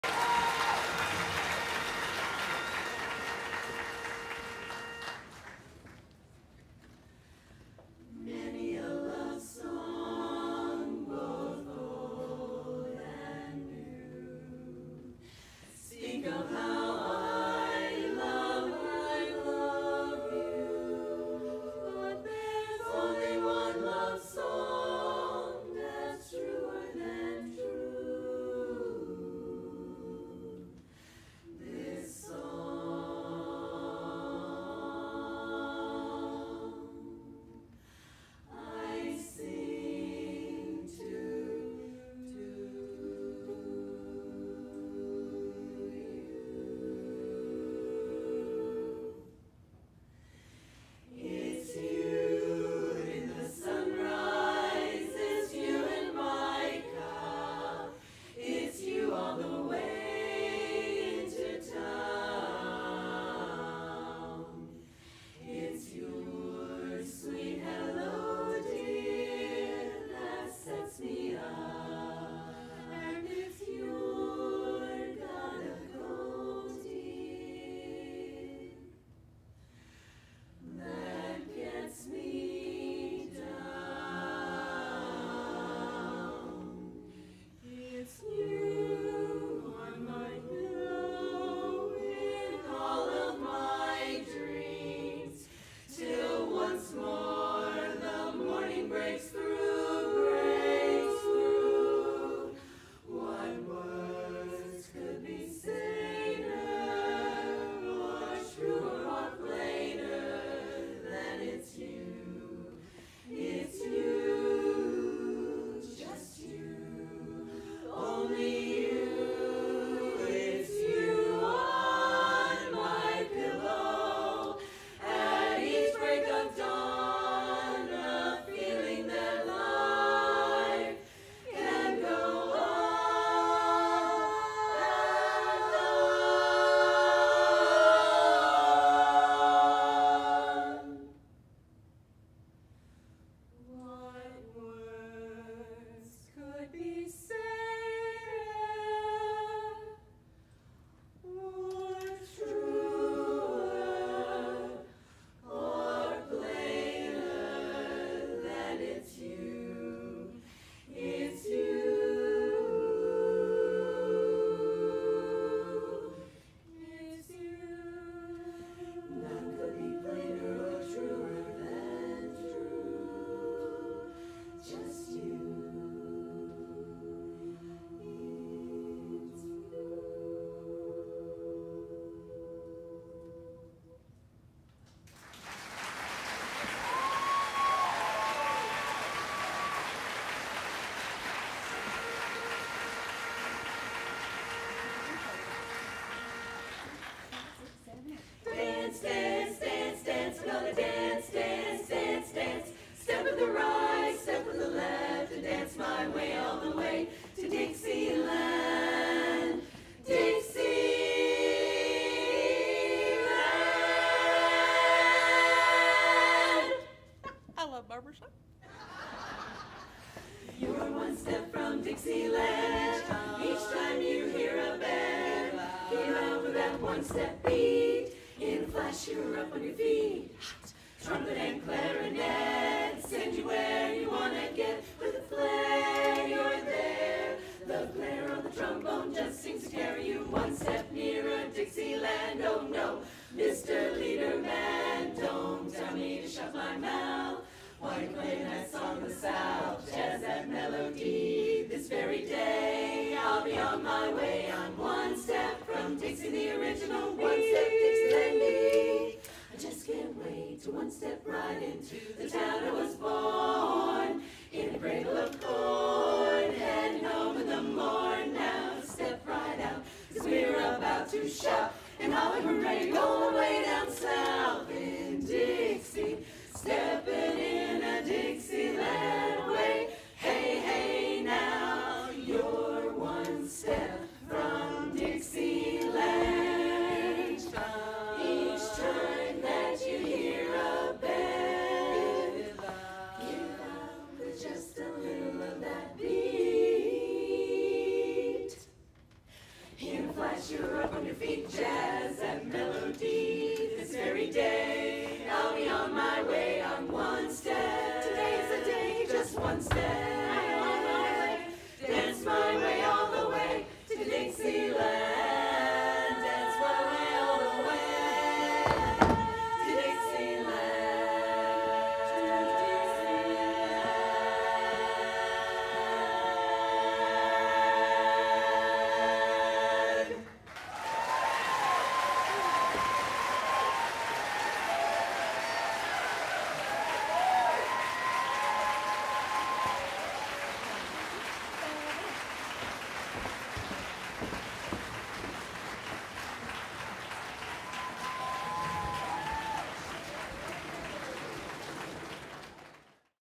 Bluegrass Harmony - 2017 International 10th Place Chorus
Representing Area 4 from Louisville, Kentucky, under the direction of Pamela Ringo, Harmony, Incorporated is proud to present the 2017 International 10th Place Chorus Champions - Bluegrass Harmony

2017 Harmony, Incorporated International Chorus Finals - Top 10

Performance recorded at the 2017 International Convention and Contests in Halifax, Nova Scotia

HI video license 201701_021 Any other use is prohibited by Harmony, Inc.

Video courtesy Daystar Productions, Black River, NY.